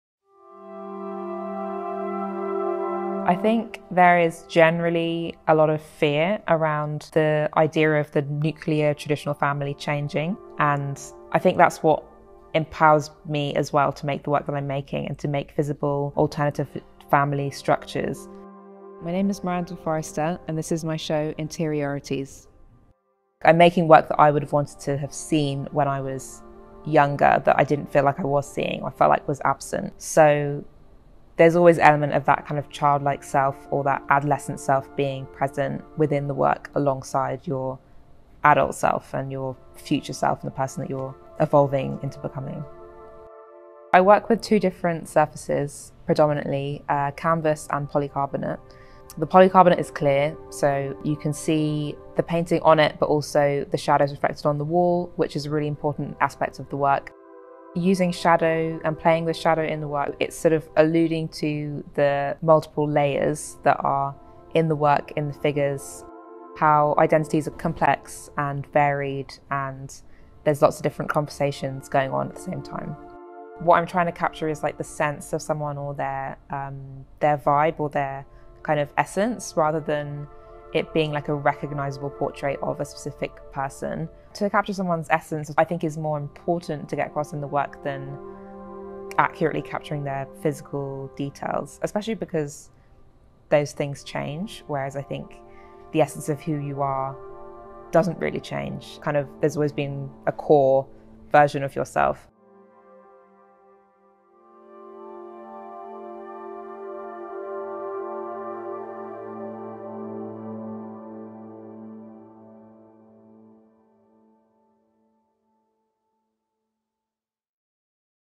Miranda Forrester | Interiorities
Mariane Ibrahim is pleased to present Interiorities, a solo exhibition by London-based artist Miranda Forrester that marks the artist’s first solo exhibition in the United States.

"Miranda Forrester paints instability as her figures waver at the edge between presence and disappearance, between figuration and abstraction. Her figures are liquid and incomplete bodies made from painterly outlines, tenuous gatherings of brushwork, and quick flashes of color. Like the queerness that they are informed by, they are painted in a language of fluidity, complexity, and mutability. Yet, despite the sense of almost frenetic precarity with the figures are rendered, Forrester’s scenes resonate with certain calm and quietude."

Excerpted text by writer and critic Zoë Hopkins